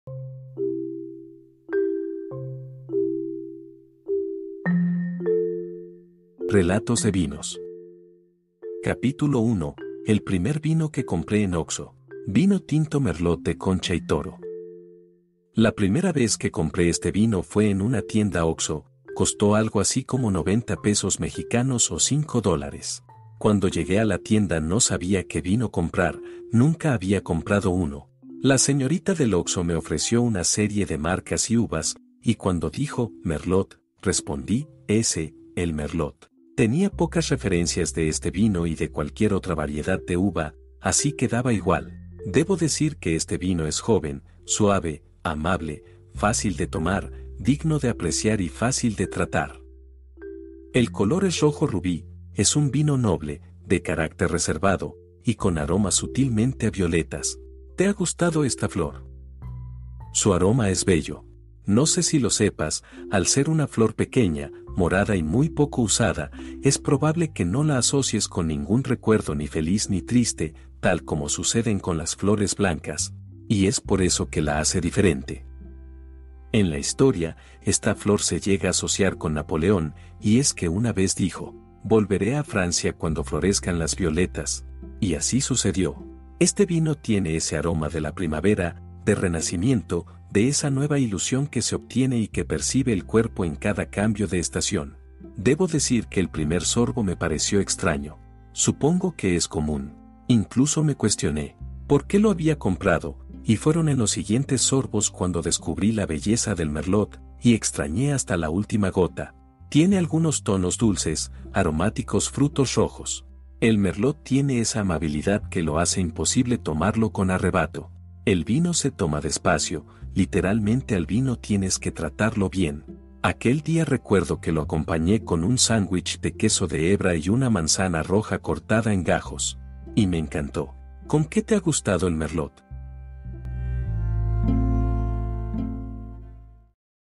Podcast Relatos de vinos Capitulo 1 El primer vino que compre en Oxxo Reservado Concha y toro Merlot
Podcast 1 de 16 Relatos de vinos
Primer vino comprado en Oxxo